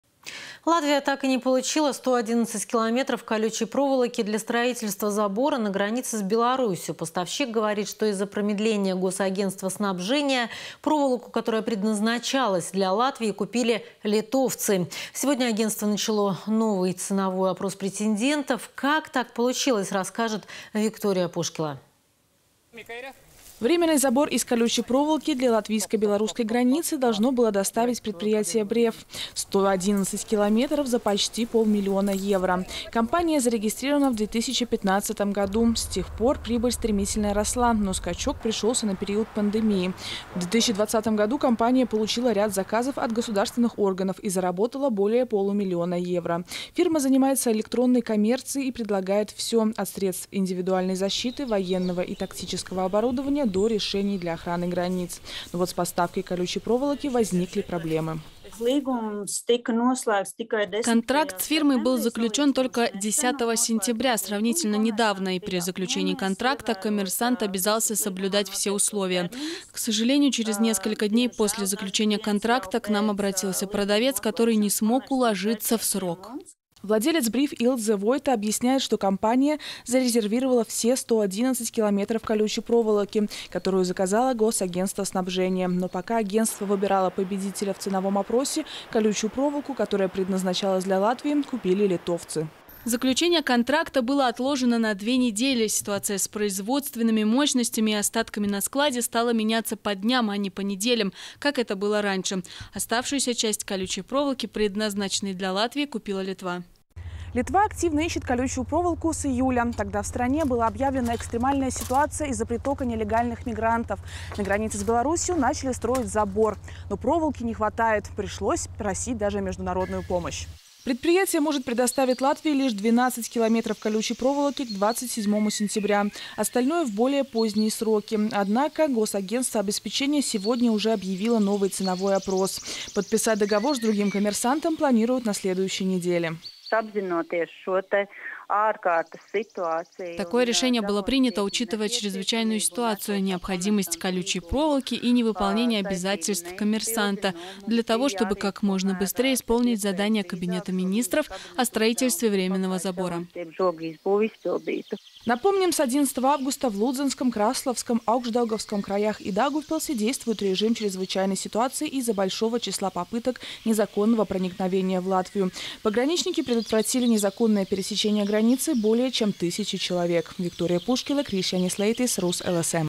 Строительство забора на восточной границе задерживается
Латвия так и не получила 111 километров колючей проволоки для строительства забора на границе с Беларусью. Поставщик говорит, что из-за промедления госагентстсва снабжения, проволоку, которая предназначалась для Латвии, купили литовцы. Сегодня агентство начало новый ценовой опрос претендентов. Как так получилось, расскажет Виктория Пушкеле.